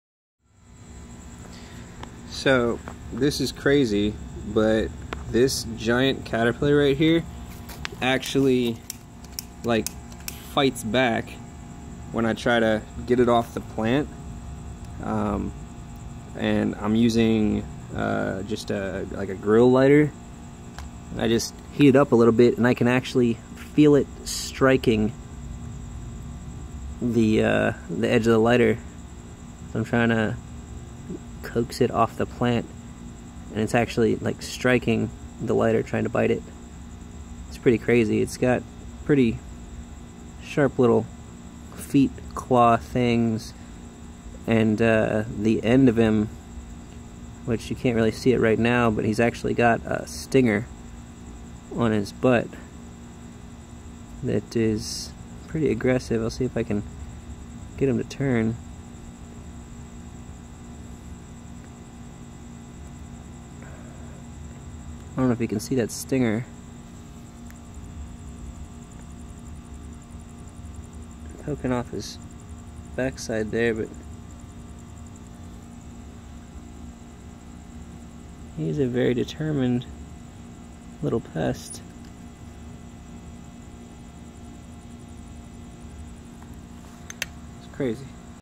Hummingbird moth caterpillar
Fighting me as I stop him from killing my pepper plants